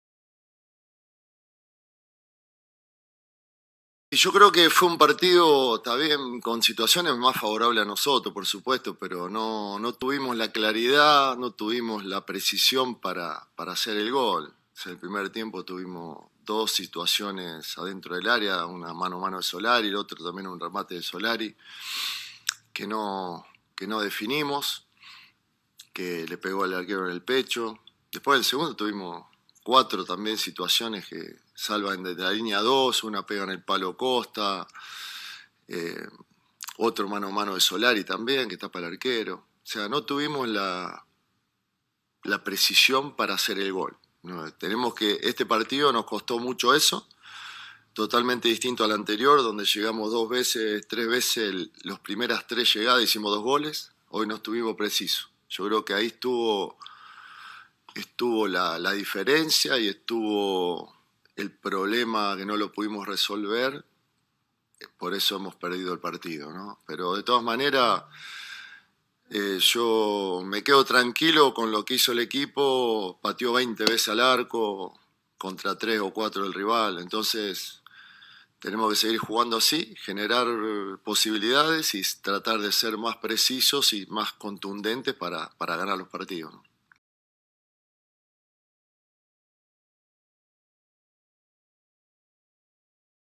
La sinceridad de Quinteros tras la derrota ante Ñublense: "Nos falta precisión"
Colo Colo cayó derrotado por la mínima ante Ñublense y, pese a que sigue puntero, hizo mucho más estrecha la distancia existente entre ellos y Unión La Calera, su más cercano perseguidor. Fue un partido ingrato que no dejó conforme a muchos, entre ellos, a Gustavo Quinteros.